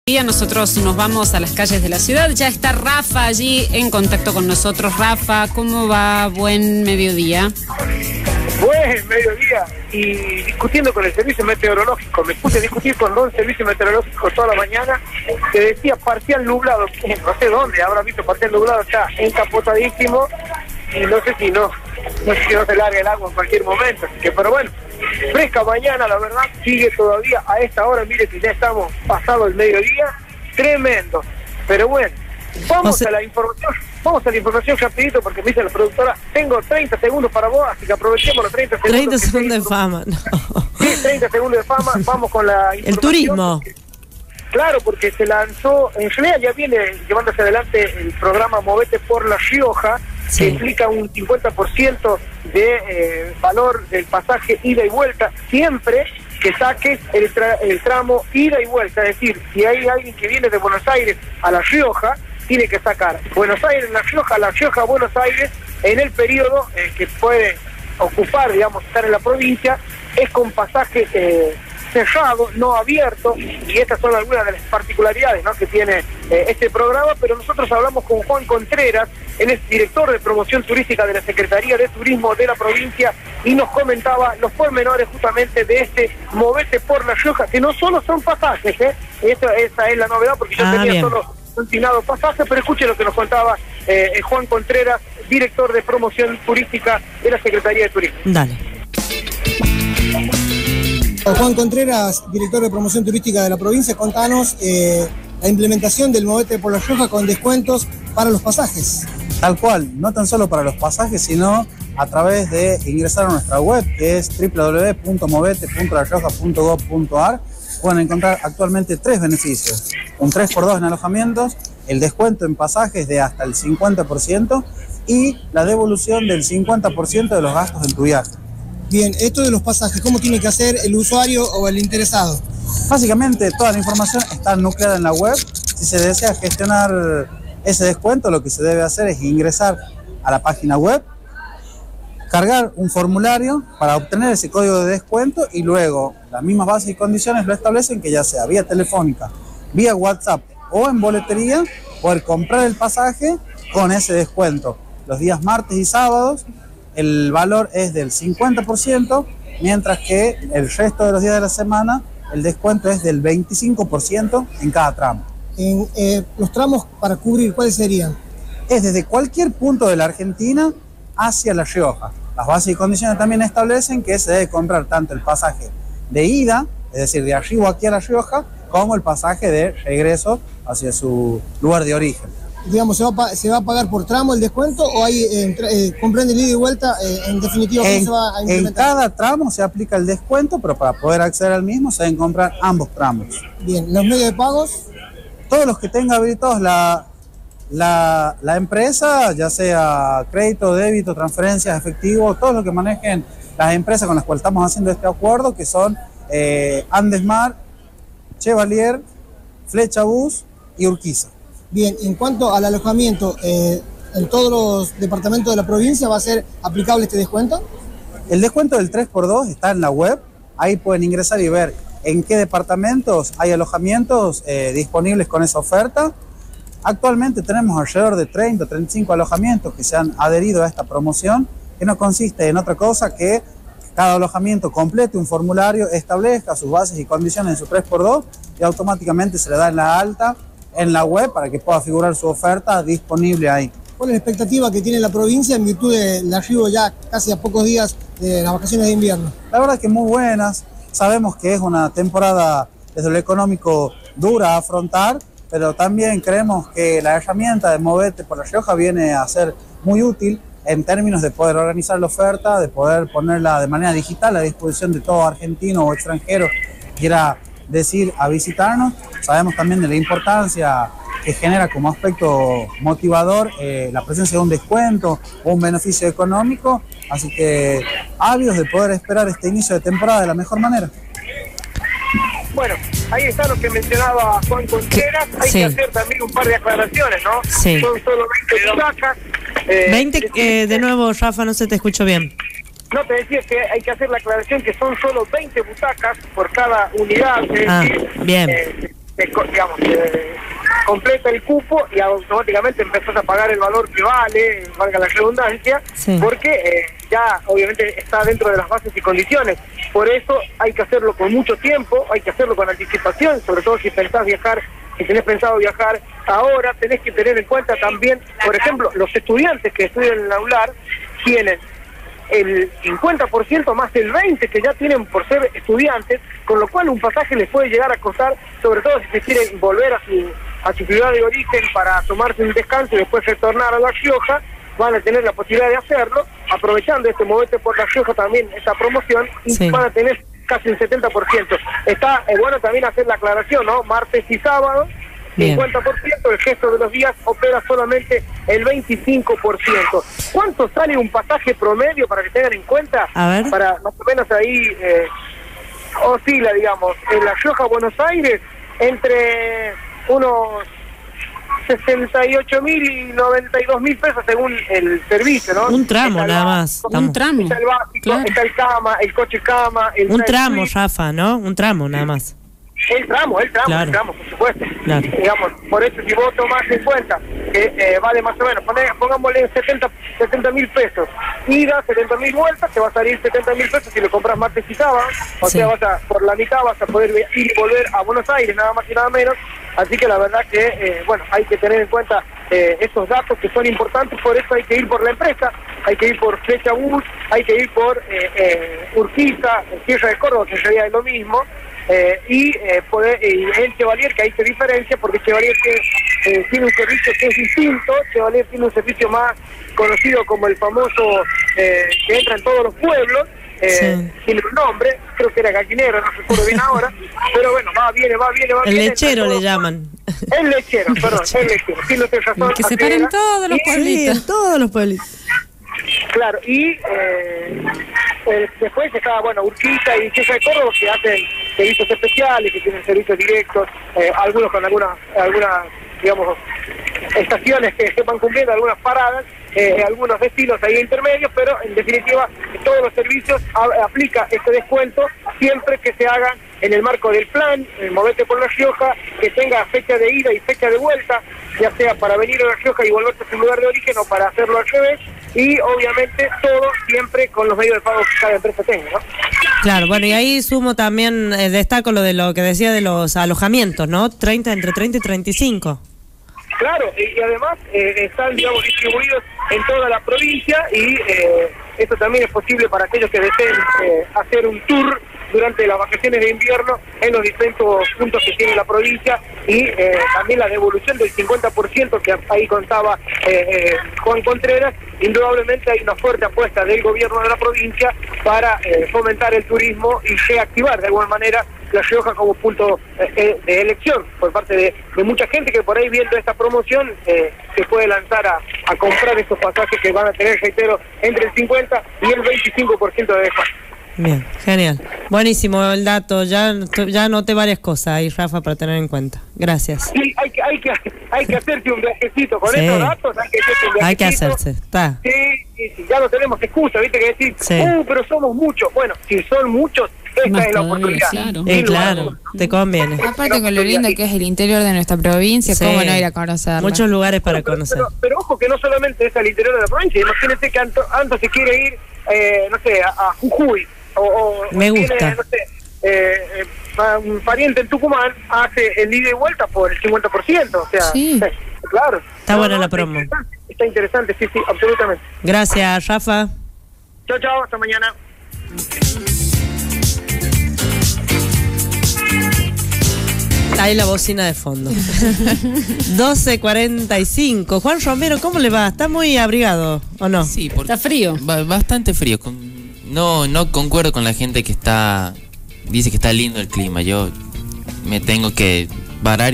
"Movete por La Rioja" ya esta en marcha
En dialogo con Multimedio UNLaR, el Director de Promoción Turística de la Secretaría de Turismo, Juan Contreras, brindó detalles de este programa implementado por la provincia que consiste en descuentos de hasta el 50% en pasajes de larga distancia, 3 x 2 en alojamiento y la devolución del 50 por ciento en los gastos del viaje.